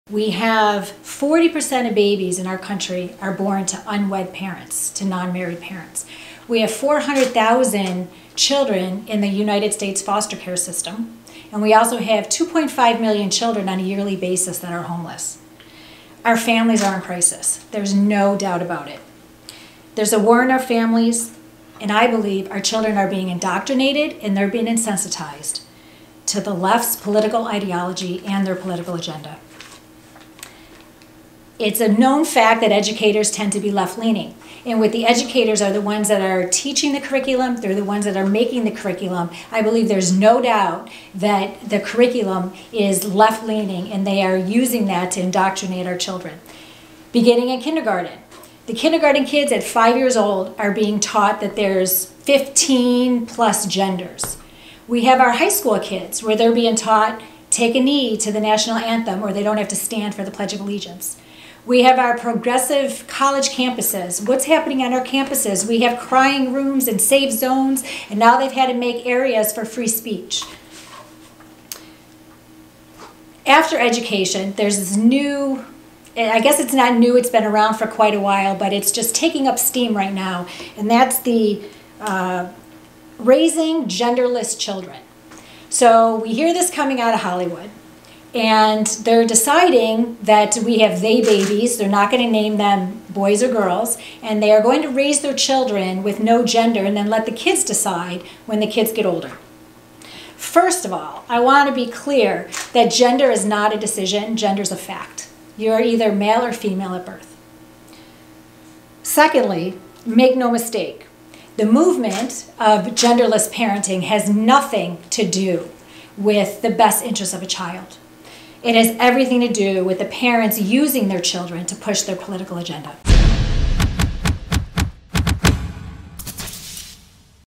'Gender Is Not a Decision; It's a Fact'
Family Law Attorney Beth Parlato explains why she believes "our children are being indoctrinated and unsensitized to the left's ideology and political agenda."